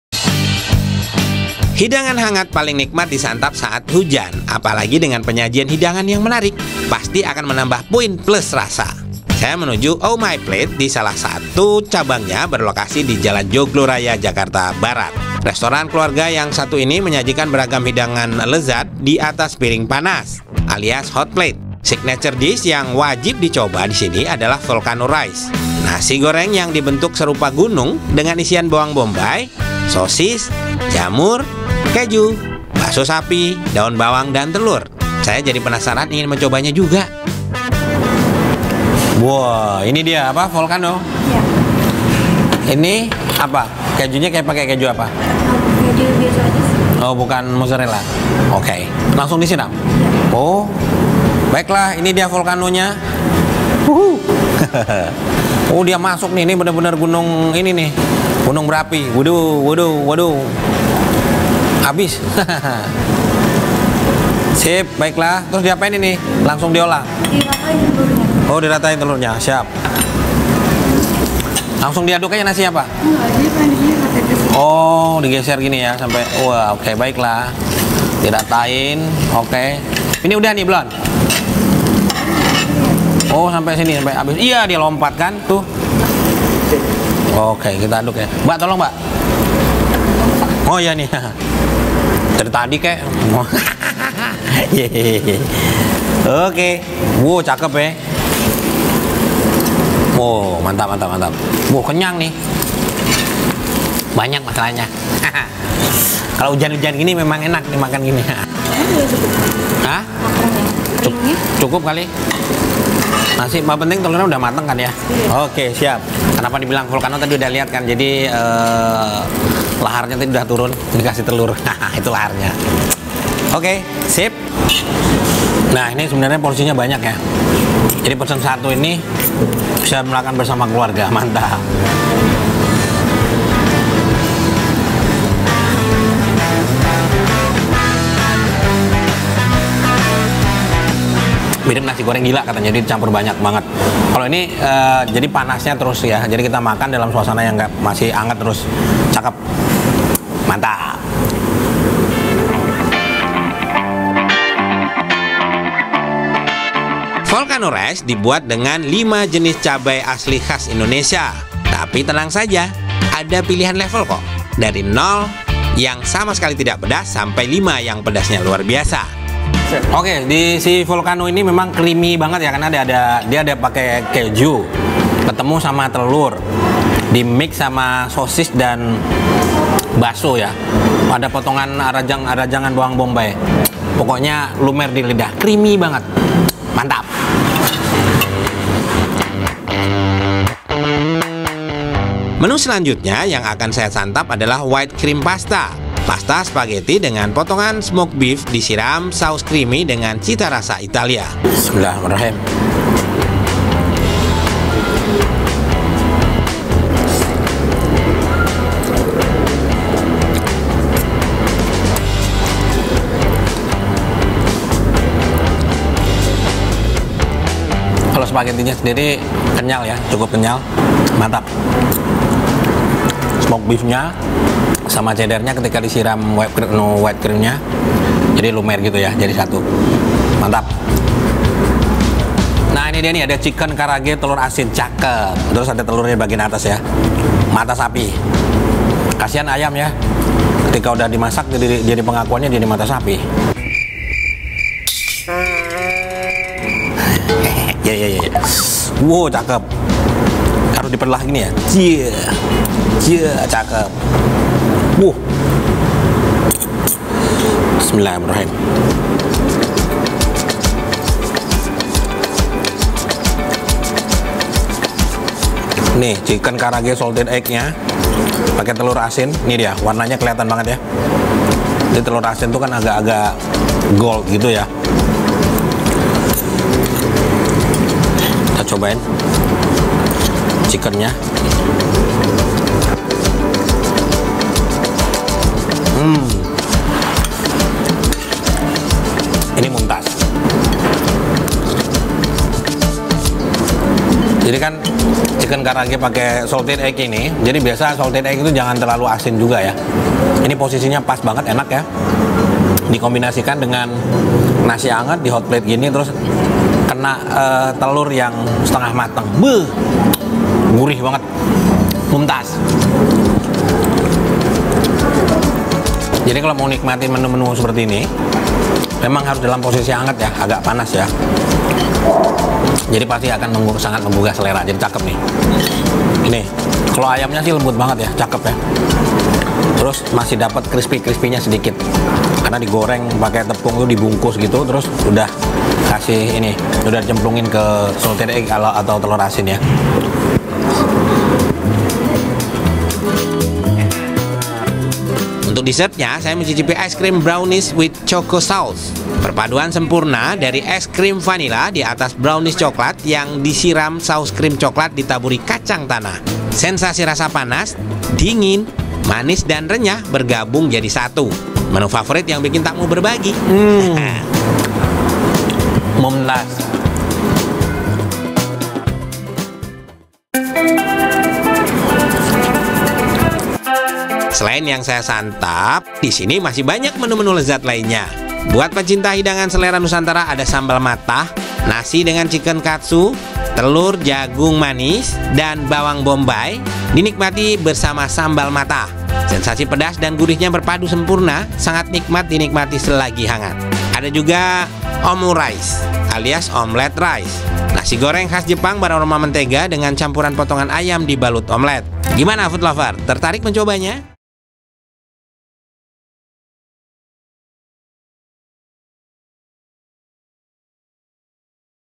Review Ow My Plate, Volcano Ricenya Creamy banget Enak!
JAKARTA, KOMPAS.TV - Hidangkan hangat paling nikmat disantap Saat Hujan apalagi dengan penyajian hidangan yang menarik pasti akan menambah poin plus rasa. Benoe menuju ow my plate di salah satu cabangnya berlokasi di Jalan Joglo Raya Jakarta Barat, restoran keluarga yang satu ini menyajikan beragam hidangan lezat di atas piring panas. Signature dish yang wajib dicoba di sini adalah volcano rice, nasi goreng yang dibentuk serupa gunung dengan isian bawang Bombay, sosis, jamur, keju, bakso sapi, daun bawang, dan telur, saya jadi penasaran ingin mencobanya juga.

Volcano rice dibuat dengan 5 jenis cabai asli khas Indonesia, tapi tenang saja ada pilihan level dari nol yang sama sekali tidak pedas sampai 5 yang pedasnya luar biasa. Di volcano rice ini rasanya creamy karena ada beberapa campuran keju, telur, bakso dan rajangan bawang Bombay, dengan porsi yang banyak pasti akan terasa kenyang, bahkan bias unuk 2 porsi.

Menu selanjutnya yang Benoe santap adalah white cream pasta, pasta spaghetti dengan potongan smoke beef disiram saus creamy dengan cita rasa Italia.

Alamat:

Jl. Joglo Raya No.59, RW.3, Srengseng, Kec. Kembangan, Kota Jakarta Barat, Daerah Khusus Ibukota Jakarta 11630